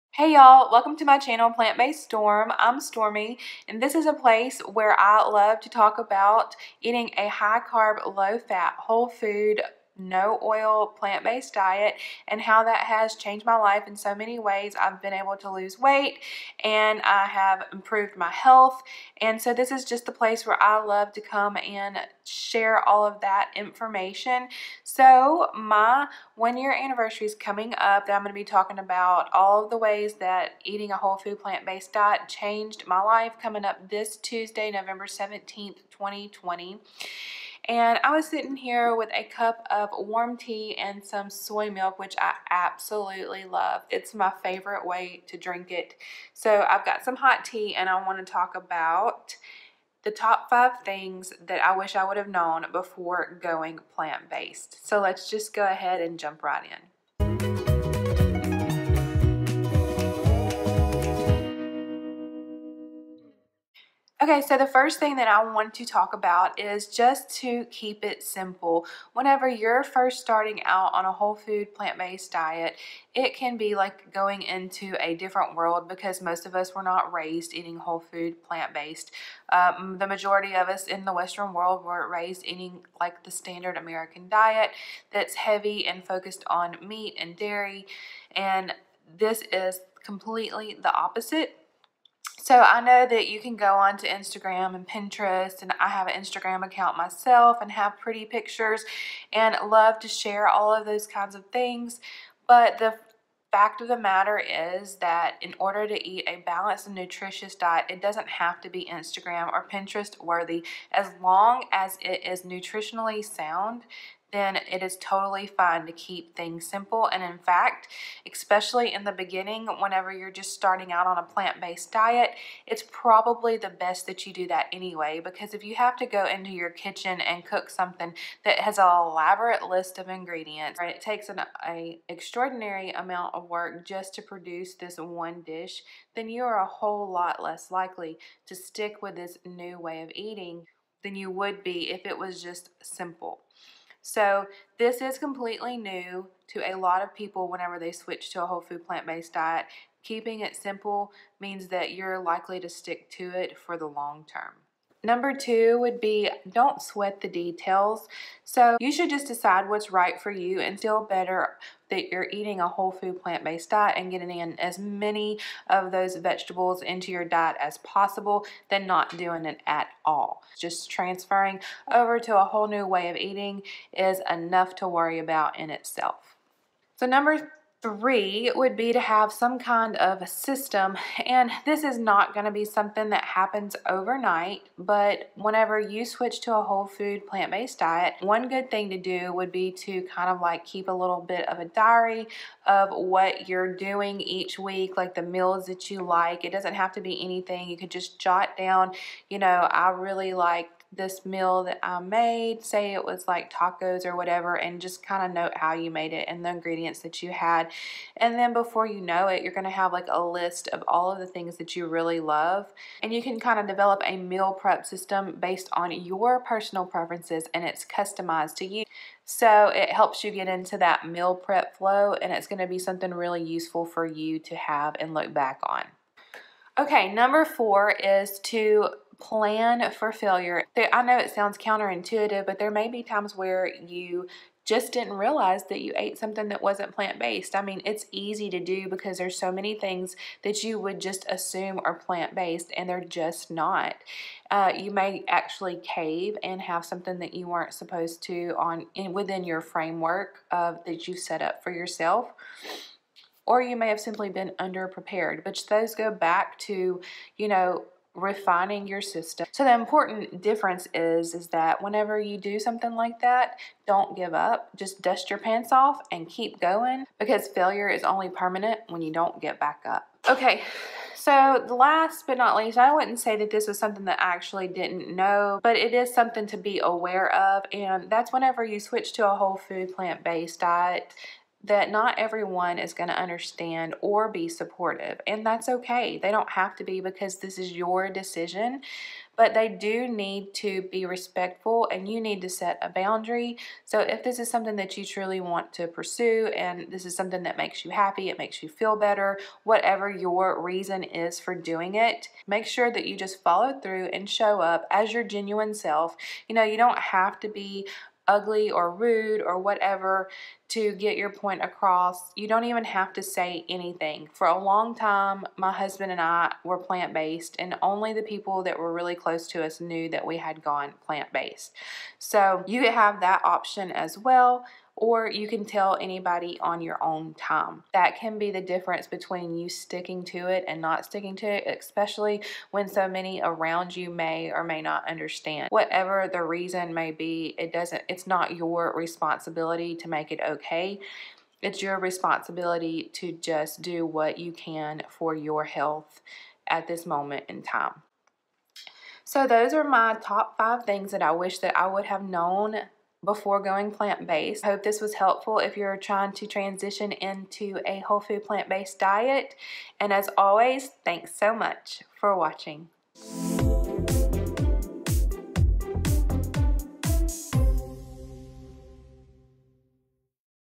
5 Things I Wish I Knew Before Going Plant Based
Hey y’all these are some real life tips and things I wish I had known before going plant based. 5 to be exact! I hope this helps you especially if you are struggling or just transitioning into the lifestyle!